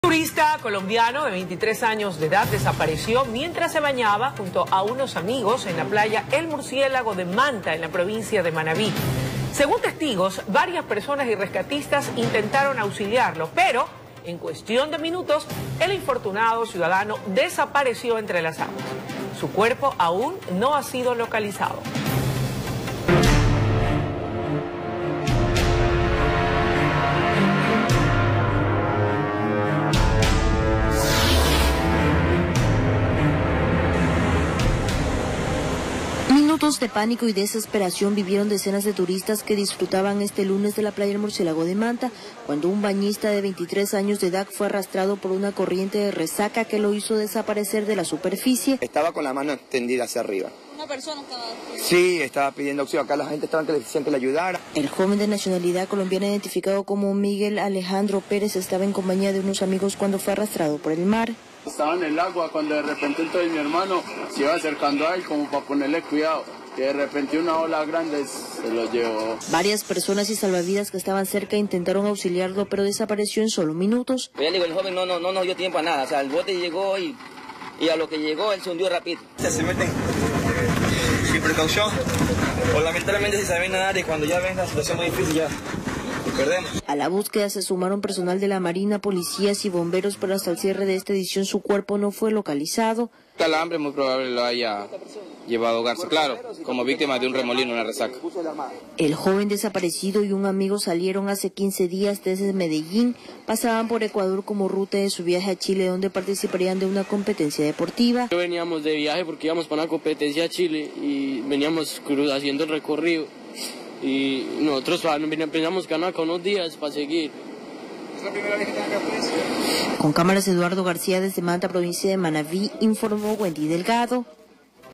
Turista colombiano desaparece mientras se bañaba en una playa de Manta
El cuerpo no ha sido encontrado

Testigos relataron que personas trataron de ayudar al joven de 23 años, sin embargo en cuestión de minutos desapareció.